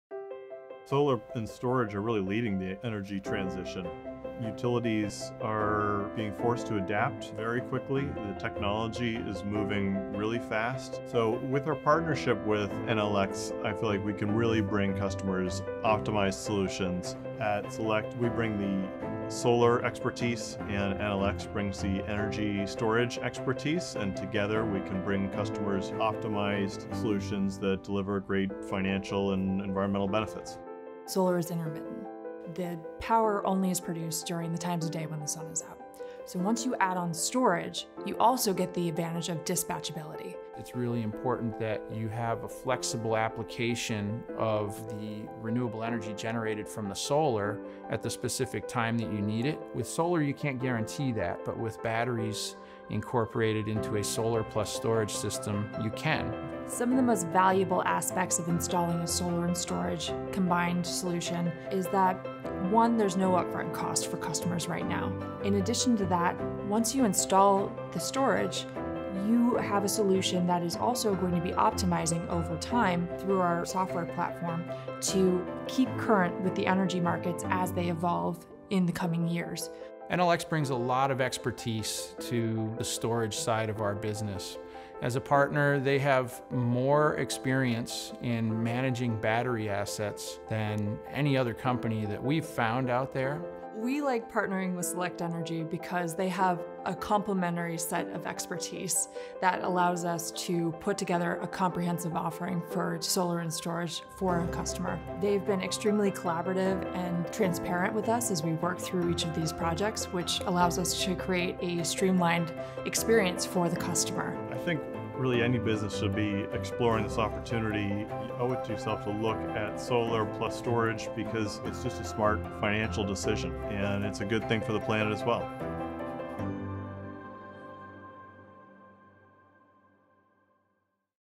The Solect Enel X Partnership
The Solect and Enel X partnership provides optimized solar and energy storage solutions that enable customers to take advantage of substantial financial and environmental benefits.